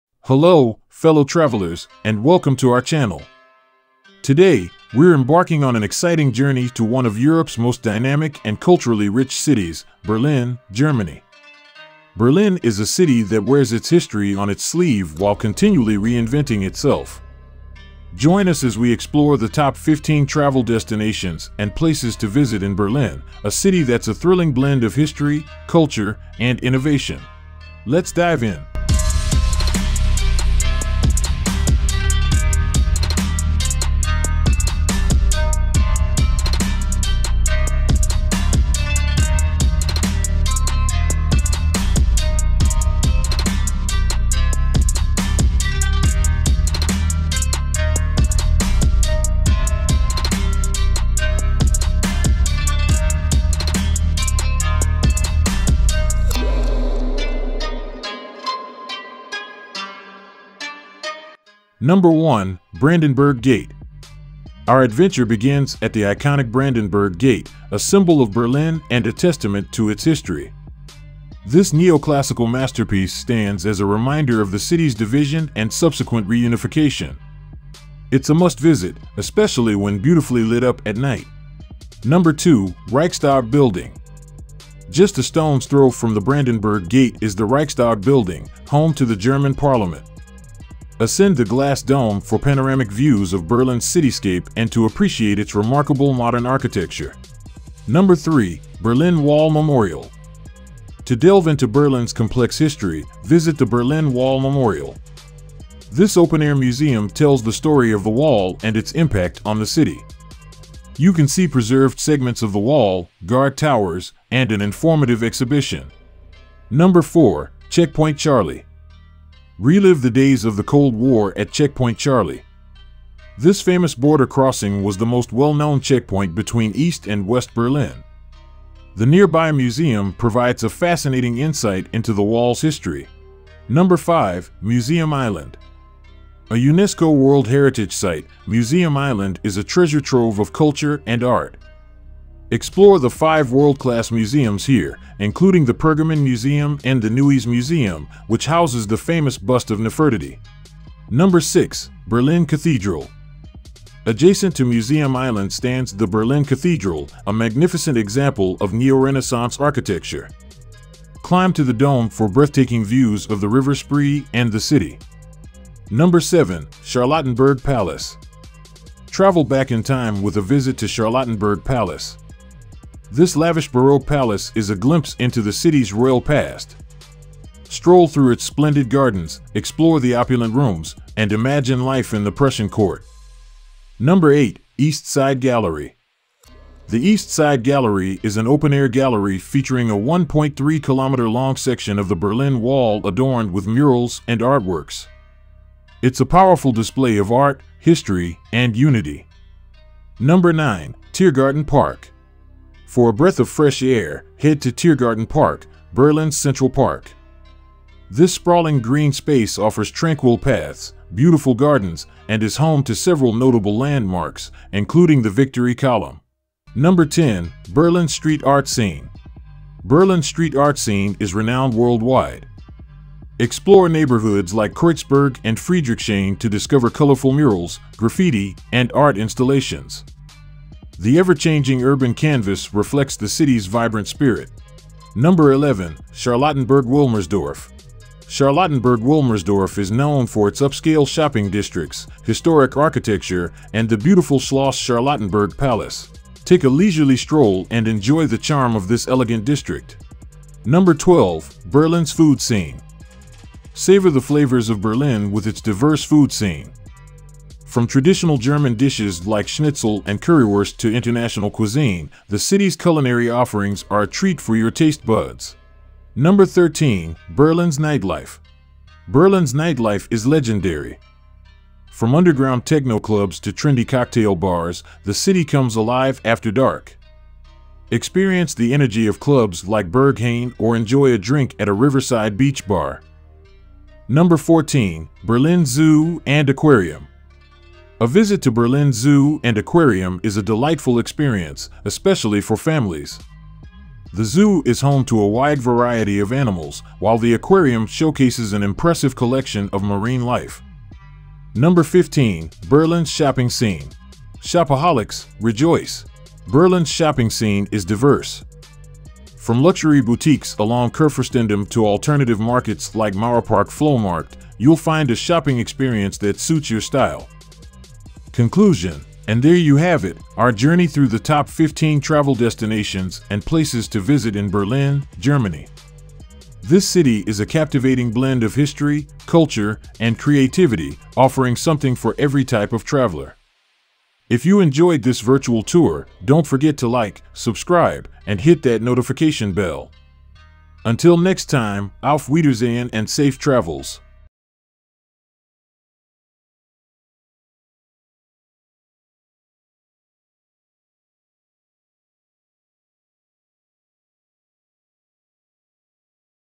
Top 15 Travel Destinations, Places To Visit In Berlin, Germany
Hello everyone in todays video we'll talk about the "Top 15 Travel Destinations, Places To Visit In Berlin, Germany"

Today, we're embarking on an exciting journey to one of Europe's most dynamic and culturally rich cities – Berlin, Germany. Berlin is a city that wears its history on its sleeve while continually reinventing itself. Join us as we explore the top 15 travel destinations and places to visit in Berlin, a city that's a thrilling blend of history, culture, and innovation. Let's dive  in!"

Make sure to watch this video till the end,because you don't wanna miss out on the best parts:)

Stay Safe & we'll meet you in the next one😊

Note:        This music is free to use under no copyright segment,